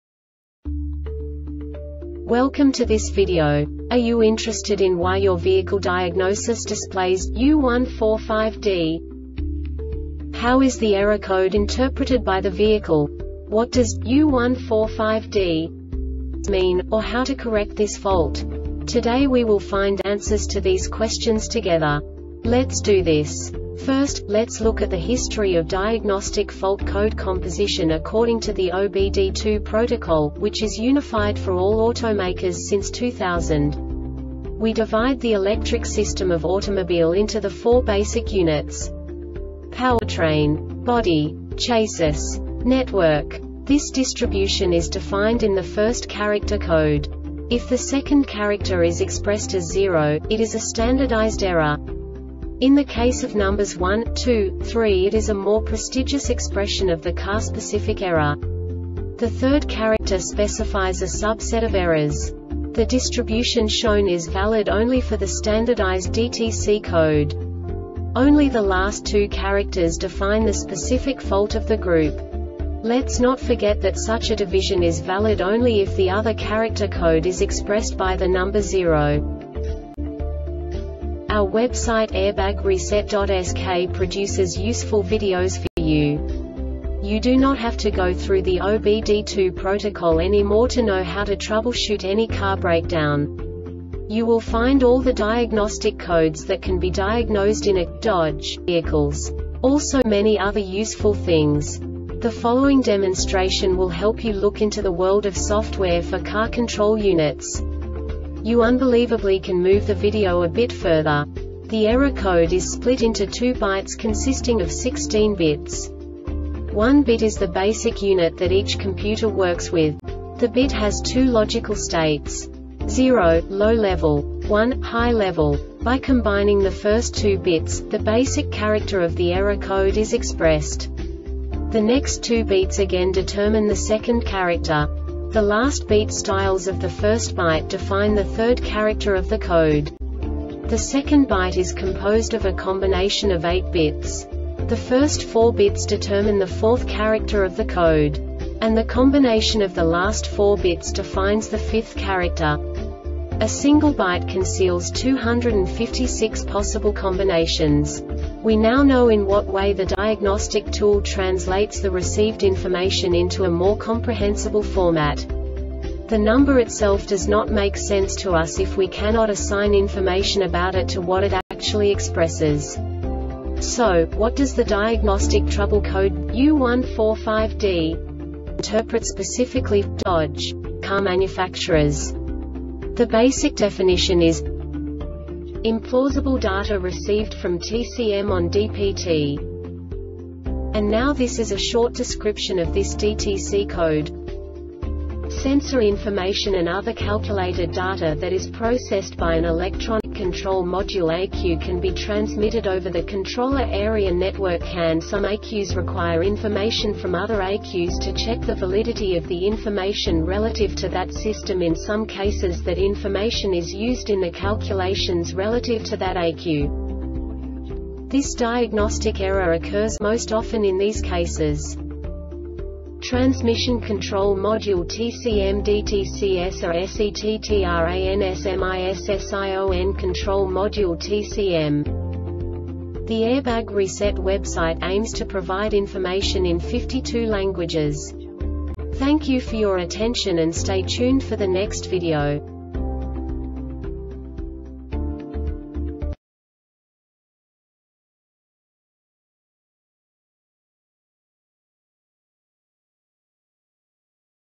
DTC Dodge U145D Short Explanation
Contents:
0:21 Basic DTC analysis according to OBD2 protocol standard.
1:48 Insight into programming
2:58 A diagnostically understandable description of the Diagnostic Trouble Code
3:05 Basic error definition

Make:
Dodge
Code:
U145D
Definition:
IMPLAUSIBLE DATA RECEIVED FROM TCM ON D-PT
Description:
Sensor information and other calculated data that is processed by an Electronic Control Module (ECU) can be transmitted over the Controller Area Network (CAN). Some ECUs require information from other ECUs to check the validity of the information relative to that system. In some cases that information is used in the calculations relative to that ECU.
Cause:
TRANSMISSION CONTROL MODULE (TCM) DTCS ARE SETTRANSMISSION CONTROL MODULE (TCM)